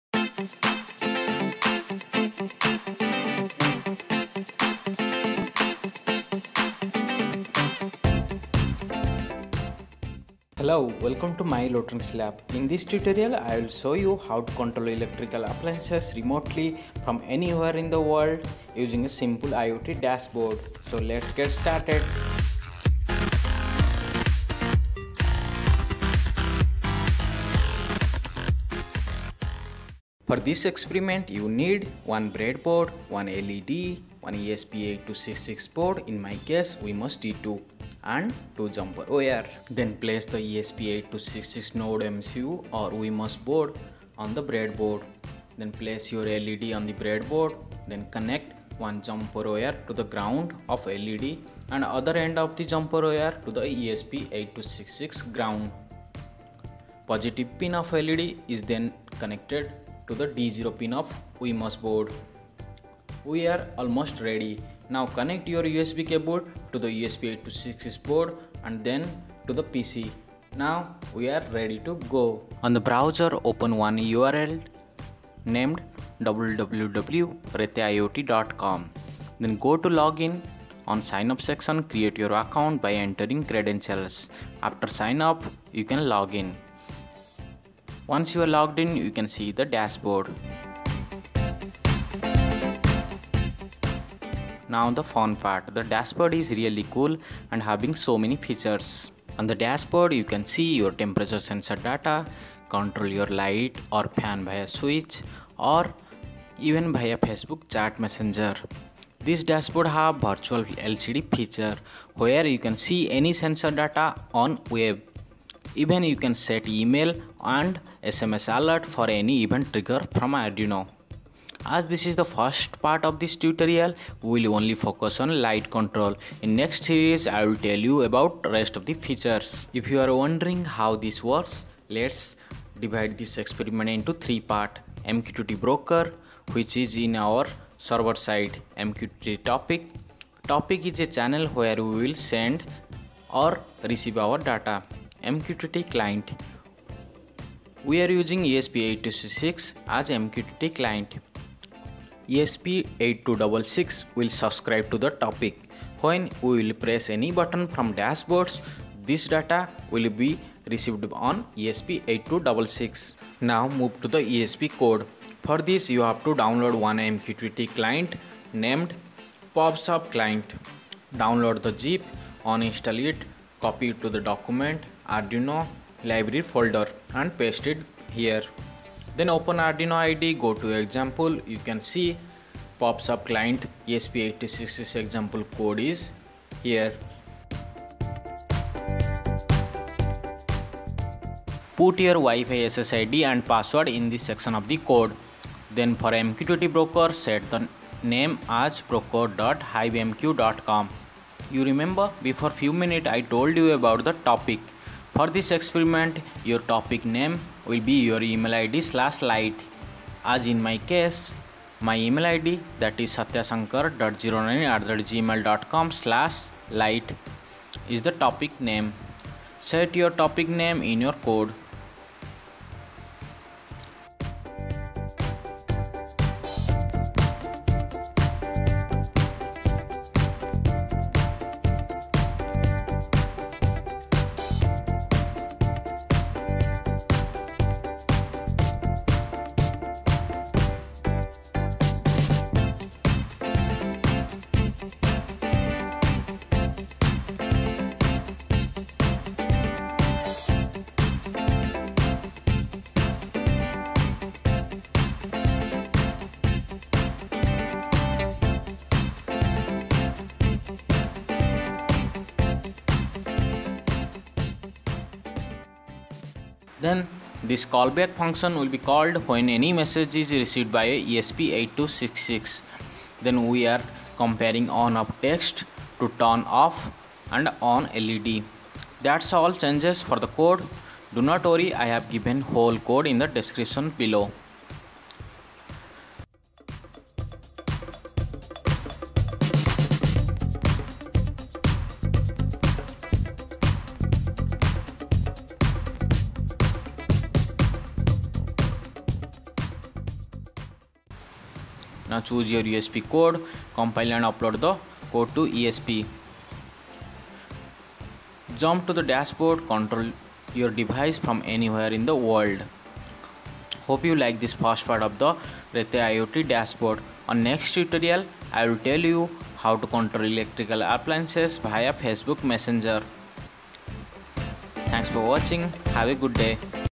Rete IOT Part 1 - MQTT ESP8266 Switch Control in just 5 Min
This video will guide you to control any electrical appliances  using esp8266 and MQTT from any where in the world in few min.

Note:
MQTT Broker Name Has been Updated from "broker.Hivemq.com" to "io.reteiot.com"

Components Used: Bread board, Wemos D1 Mini (Node MCU or any esp8266 board), LED and few jumper cable.

Hope You like this tutorial, Do let me know, If you have any question.

Thanks
My Electronics Lab